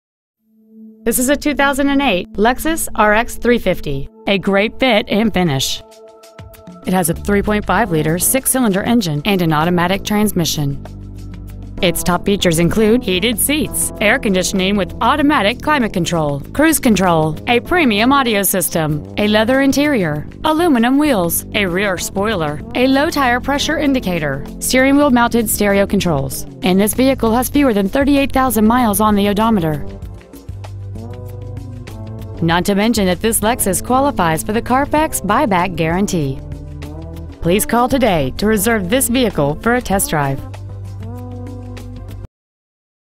2008 Lexus RX 350 Plano TX 75093
Year: 2008
 Make: Lexus
 Model: RX 350
 Engine: 3.5L DOHC SFI 24-valve V6 engine
 Trans.: Automatic
 Exterior: Gold
 Miles: 37,667
 Interior: Tan
 Stock: 5795